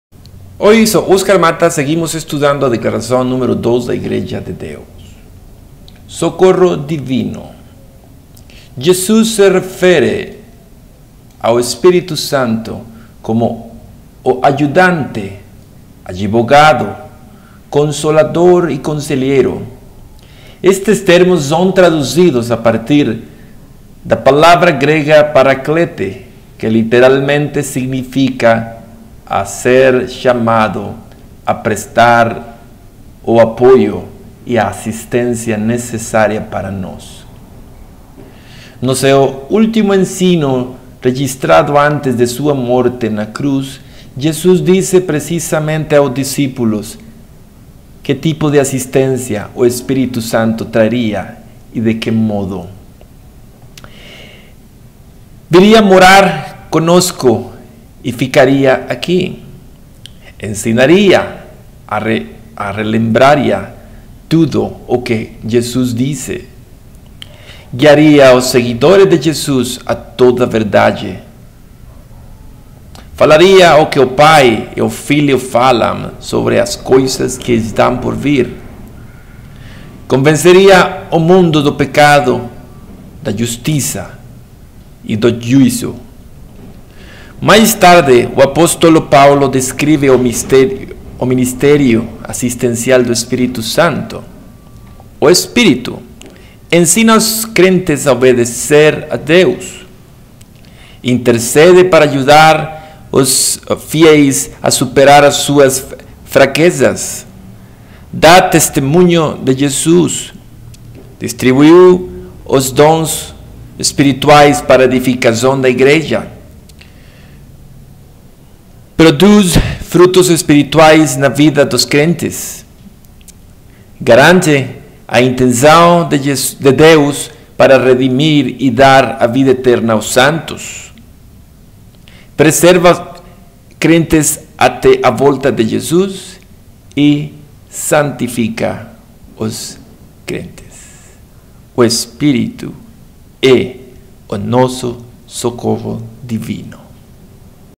Declaração de Fé 2 "A Divindade", parte 15 "O Espírito é o Socorro Divino"
Jesus se refere ao Espírito Santo como o Ajudante, Advogado, Consolador e Conselheiro. Estes termos são traduzidos a partir da palavra grega paraklete, que lite- ralmente significa “a ser chamado a prestar o apoio e a assistência necessária para nós.”